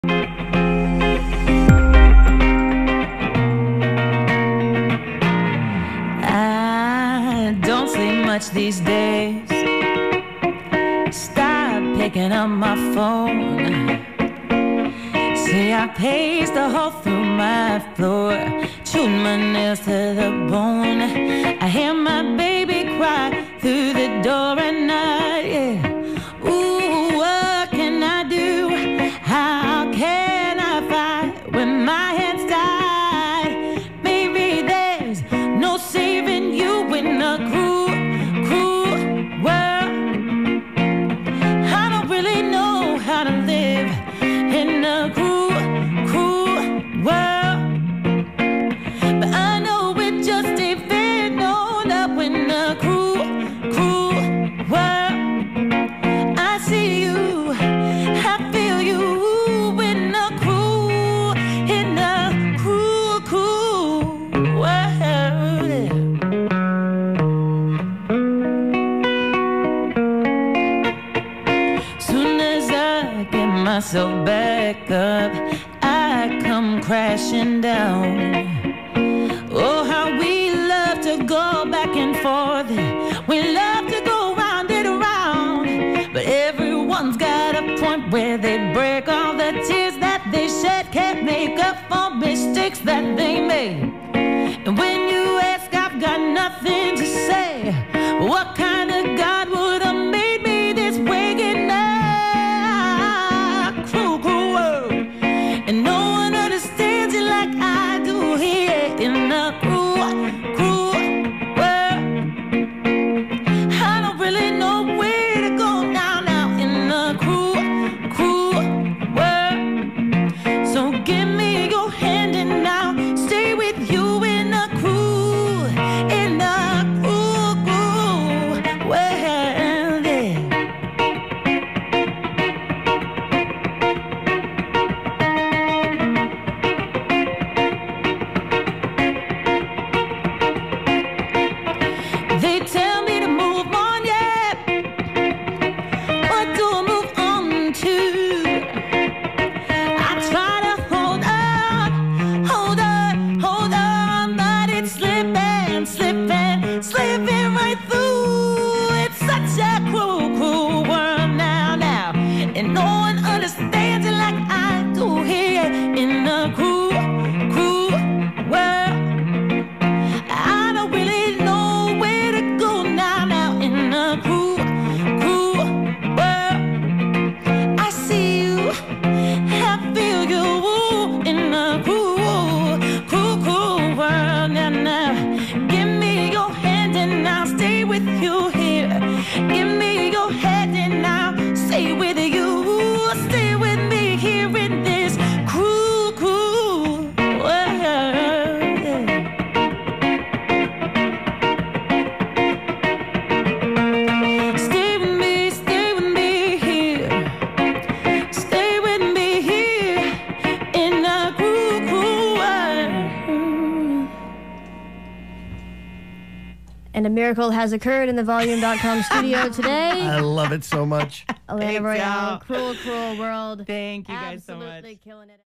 Alanna Royale - (Live) | Lightning 100 | Volume.com Studio
Did you miss Alanna Royale live from the Volume Studio at Lightning 100 last month?

The stunning singer-songwriter will win you over with her eclectic and bold sound that will stop you in your tracks.

Head over to Volume.com for more exclusive performances in the New Year!